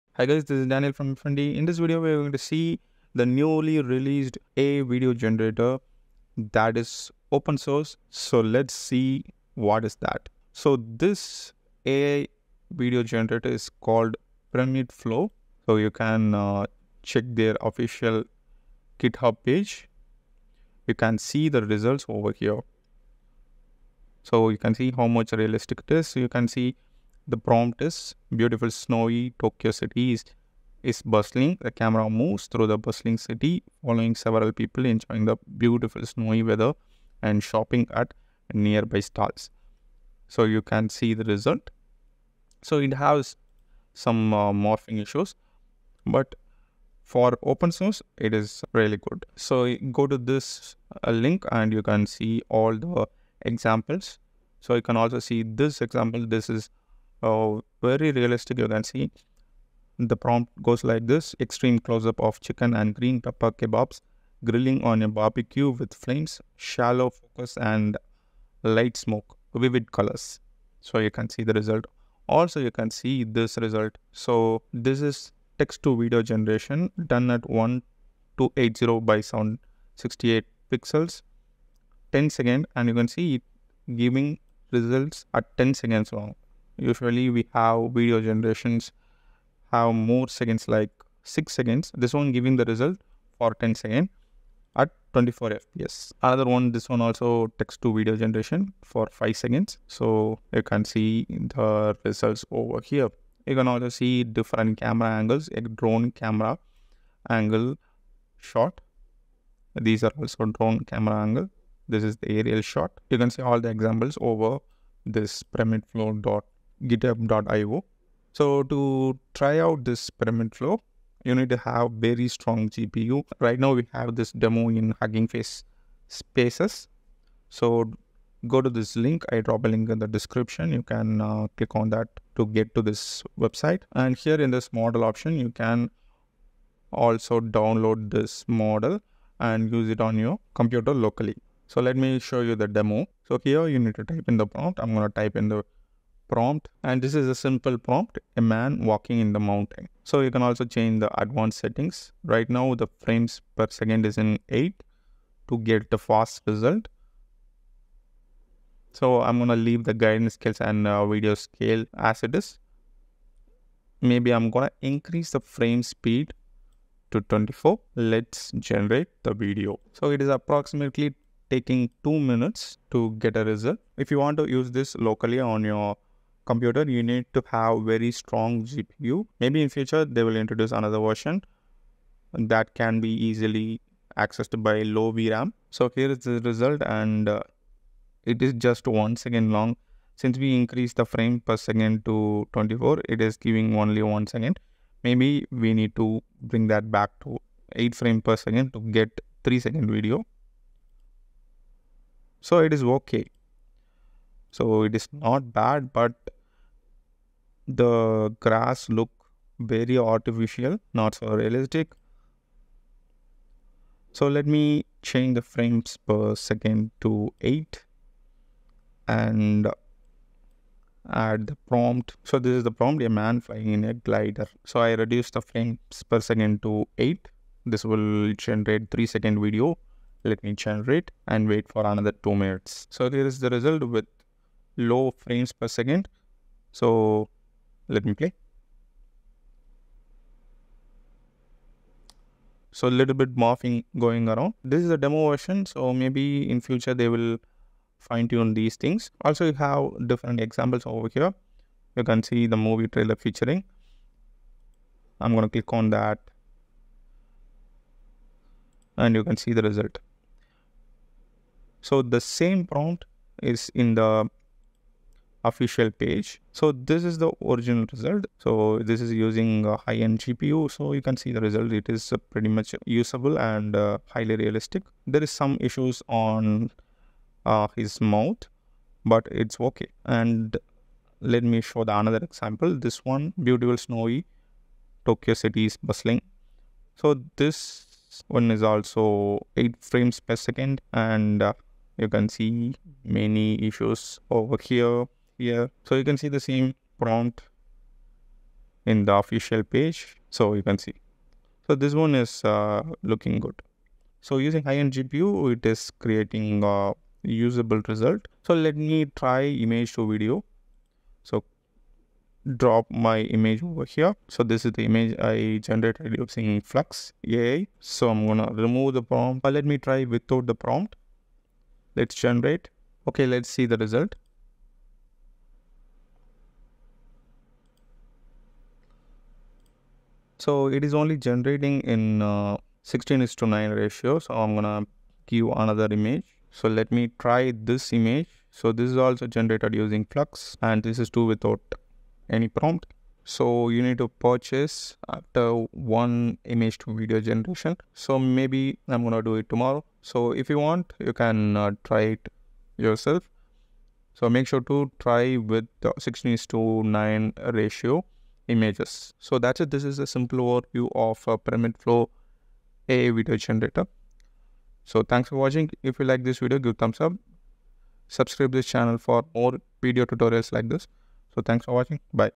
The New AI Open Source Video Generator! Pyramid Flow | How to try Pyramid Flow
Pyramid Flow - Open Source AI Video Generator

Hi Everyone, This is Daniel from f&D. In this video, We are going to see How to try the newly released AI Video Generator called Pyramid Flow